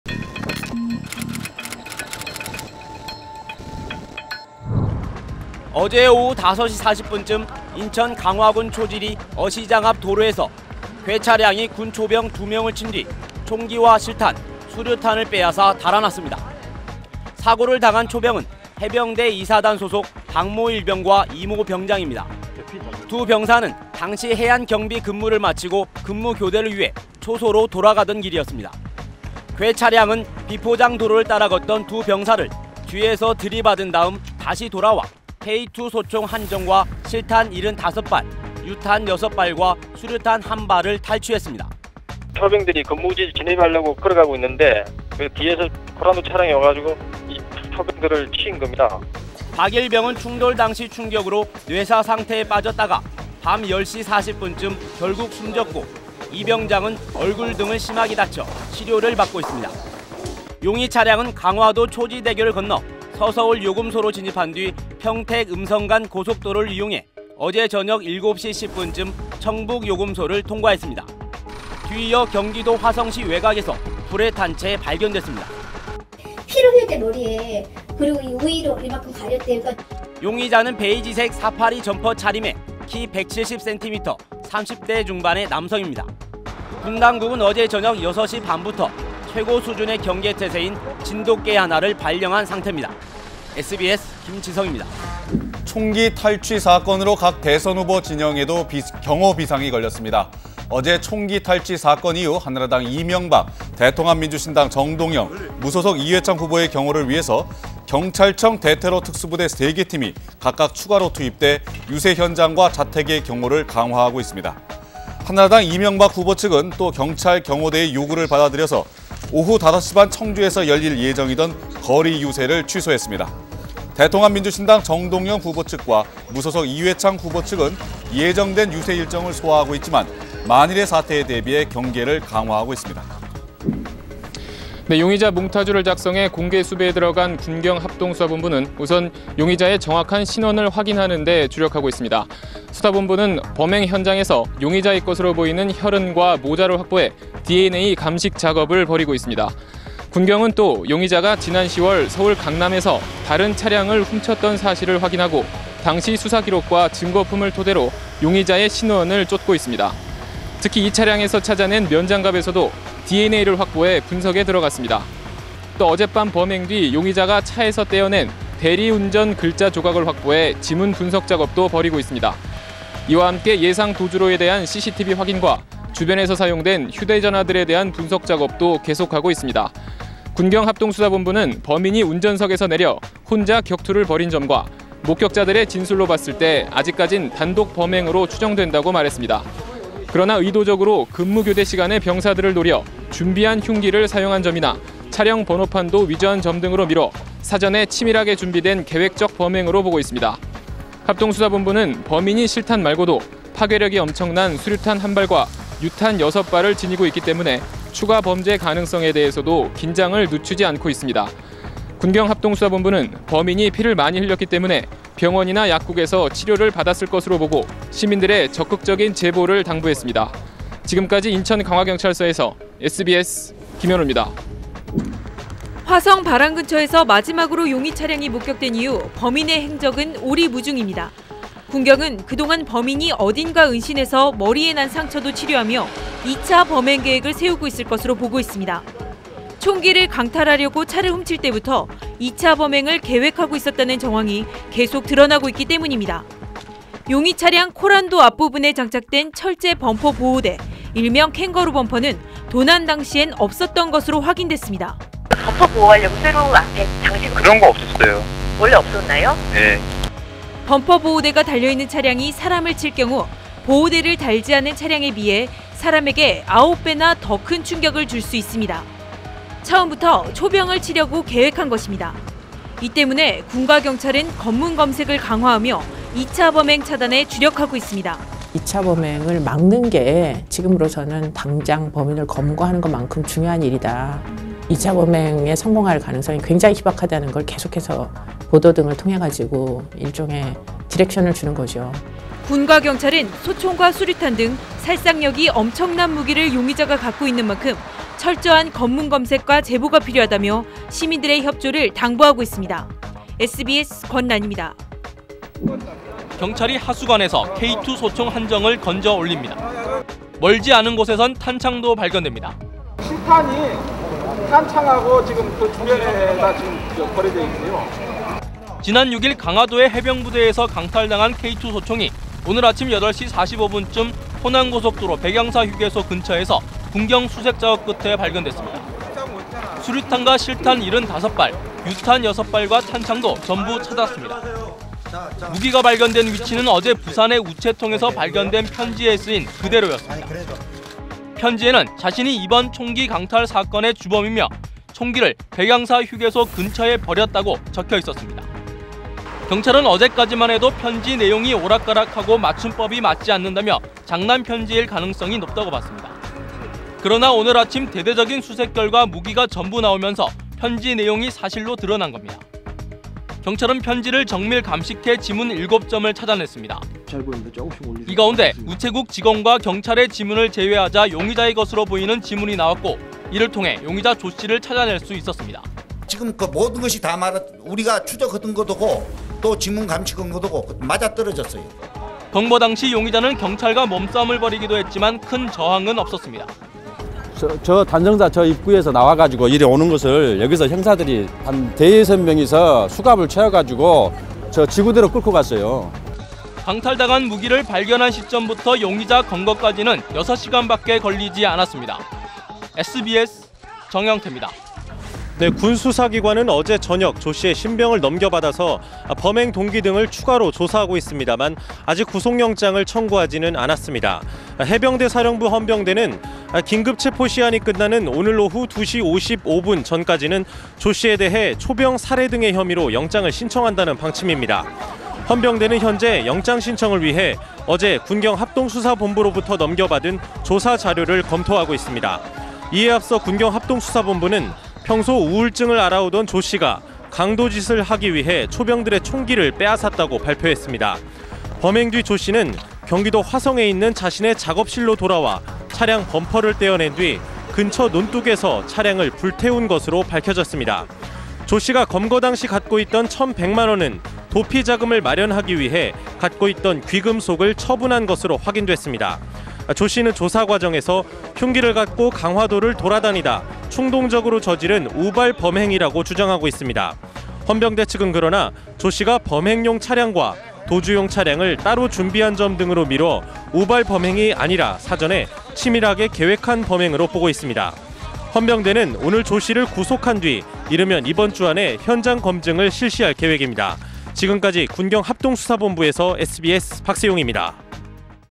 [그때는지금은] 2007년 강화도 해병대 총기 탈취 사건, 범인이 쓴 편지엔… / SBS / 모아보는 뉴스
2007년 10월 온 나라를 떠들썩하게 만들었던 강화도 해병대 총기 탈취 사건.
이 사건으로 입대 7개월 차인 박영철 일병이 숨을 거두는 안타까운 일이 있었습니다.

일주일 만에 잡힌 범인이 밝힌 범행의 이유는 황당했습니다.

지난 1월 SBS '꼬리에 꼬리를 무는 그날 이야기'에서 이 사건을 다뤘는데요. 15년 전 당시 긴박했던 뉴스를 모아봤습니다.

00:05 군인 치고 총기·수류탄 빼앗아 달아나…1명 사망
01:35 총기탈취 여파…대선후보 진영도 '경호 비상'
02:13 흉기 준비하고 번호판도 위조…계획 치밀했다
03:45 총기강탈 사전준비 치밀했다…'2차 범행' 긴장
05:05 공포의 일주일, '용의자의 편지'가 사건 풀었다
06:45 "충동적으로 저지른 우발 범행?"…의문투성이